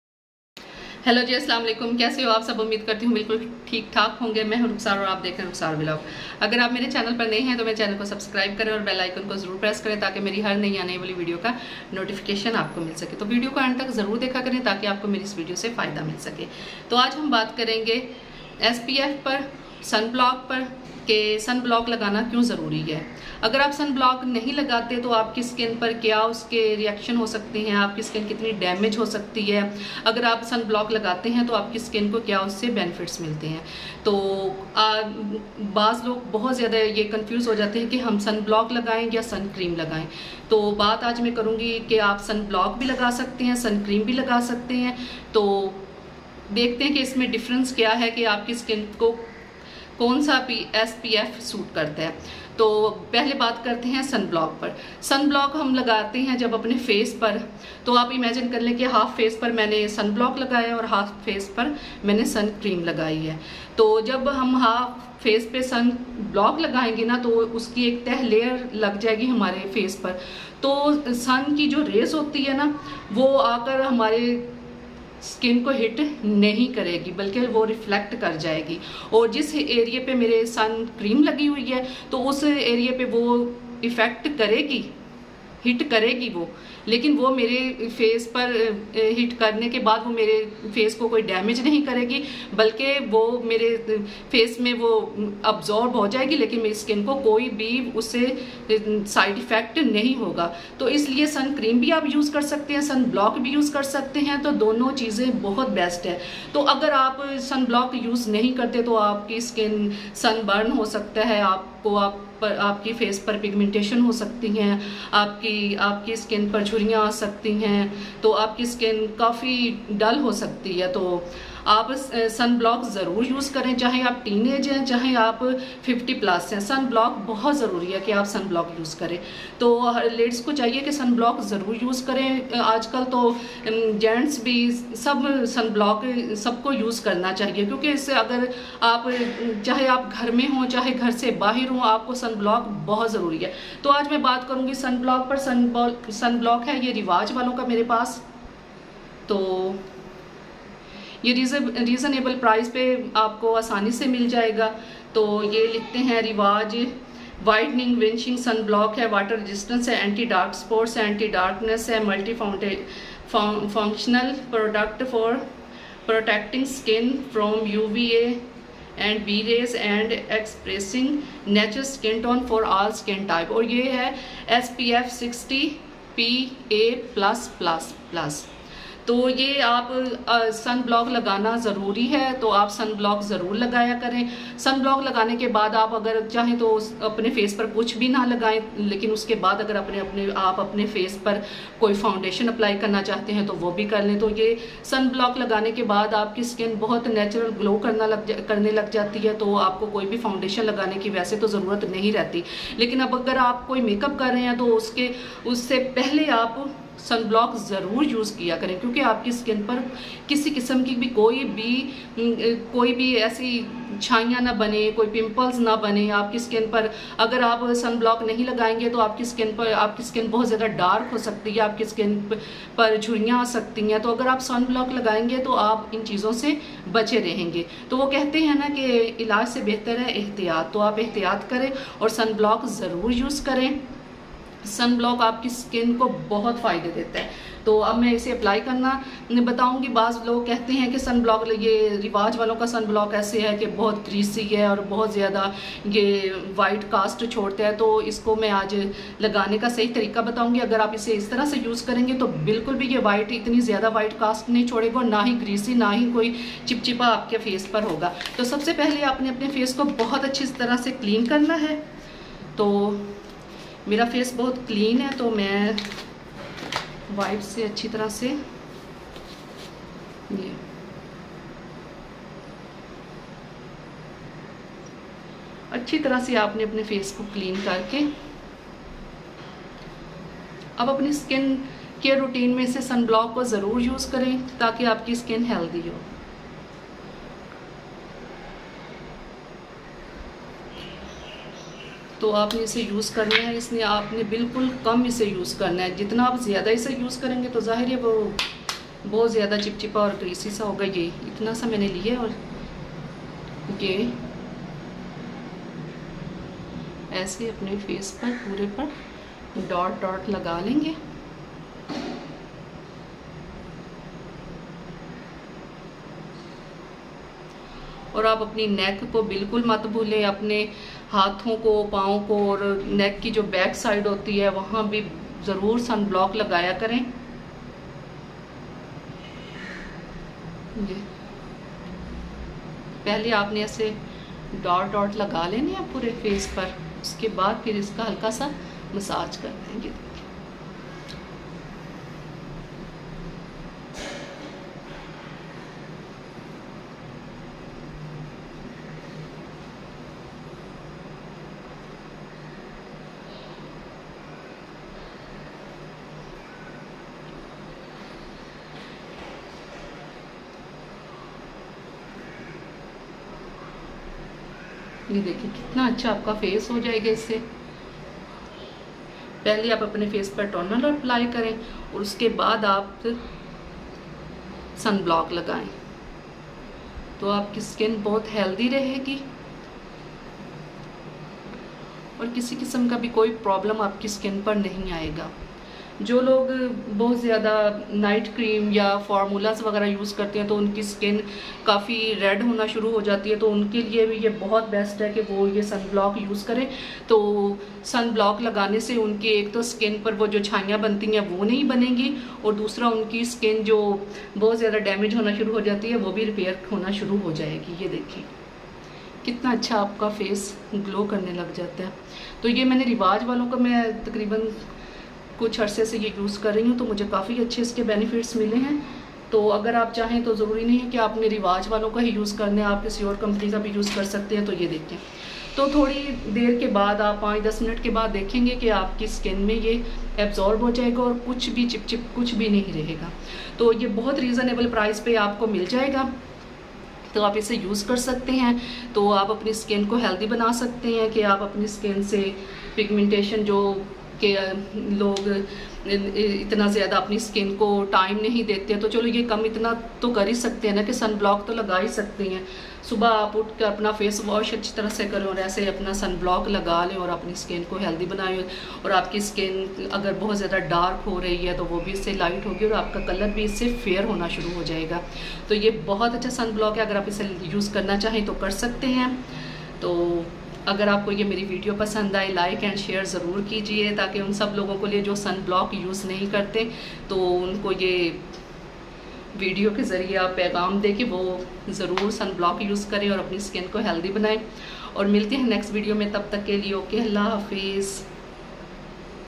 Benefits To Use Sunblock | Rukhsar Vlog
Hi, In this video I explained, the benefits to use Sunblock. I hope this video will be useful for you guys.